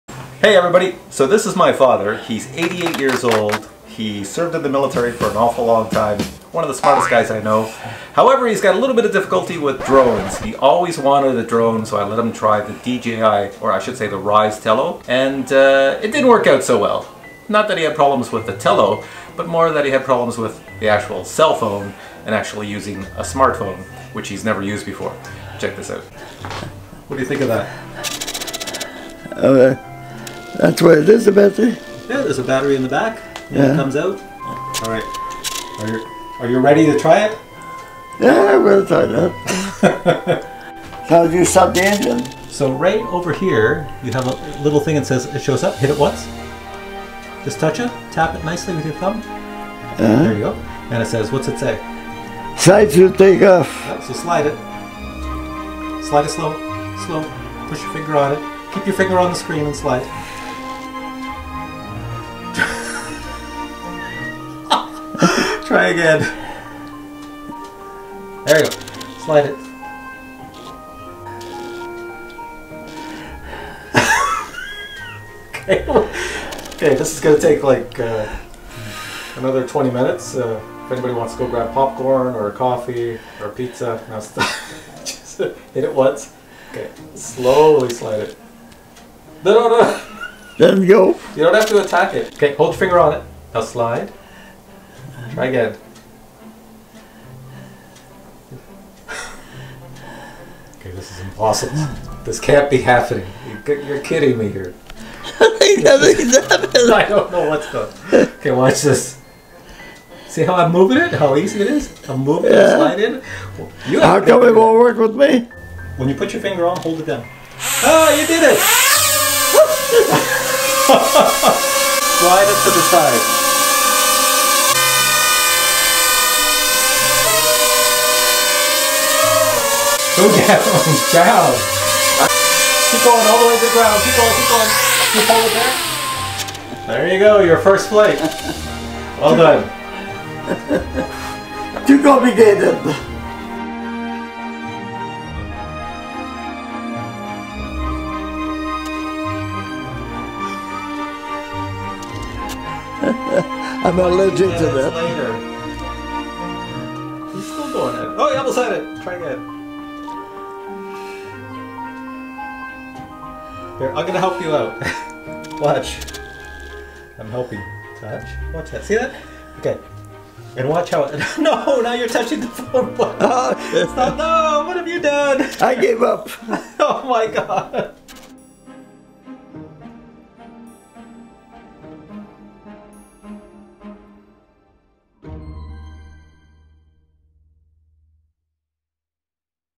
My 88 year old Father wants to fly the TELLO Drone. What could go wrong?
My Father has always wanted to fly a drone, so I thought the RYZE DJI TELLO drone would be perfect for him.  Apparently his touch screen skills are rather non-existent.

MY GEAR USED TO CREATE THIS VIDEO:
Canon 80D
BIG Gorillapod